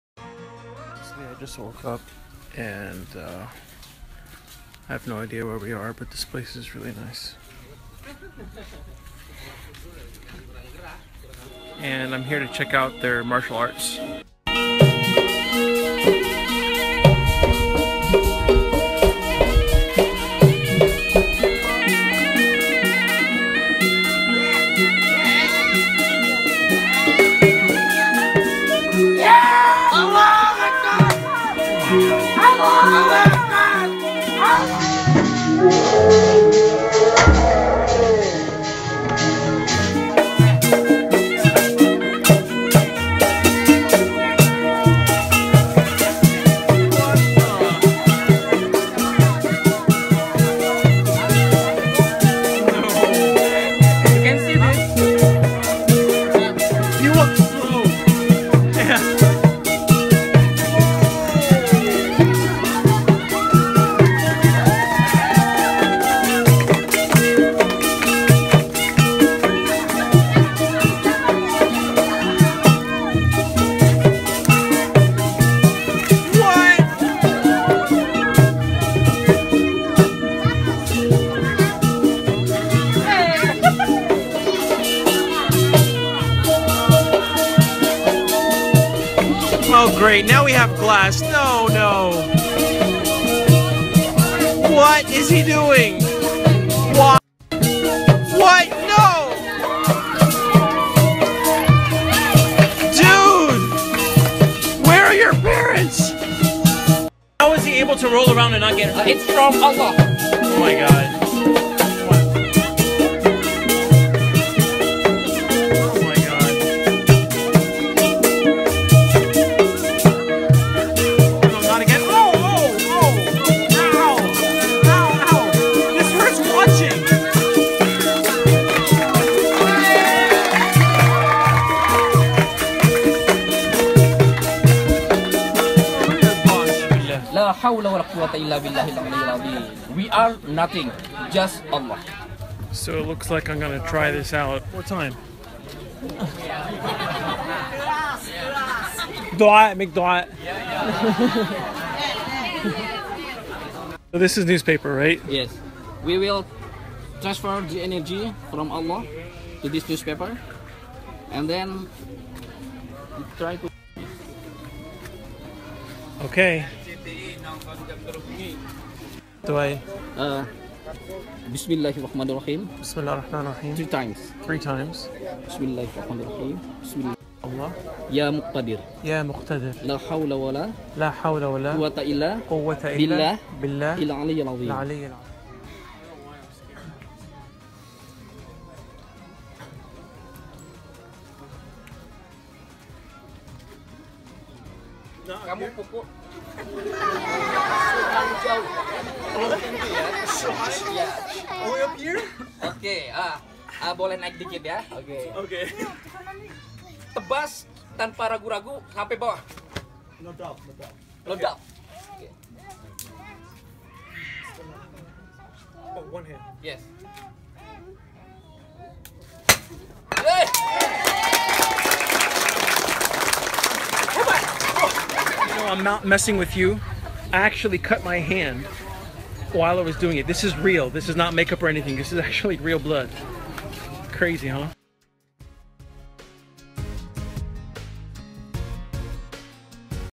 Raef's SNAPCHAT Vlog: Debus Martial Arts - Fire Walking in Banten
Pangeran Sukma Jati gave me a tour of his Debus Martial Arts school - They train the body to tolerate and withstand unbelievable amounts of stress and pain. Watch how I am able to break a metal bar with just a roll of newspaper!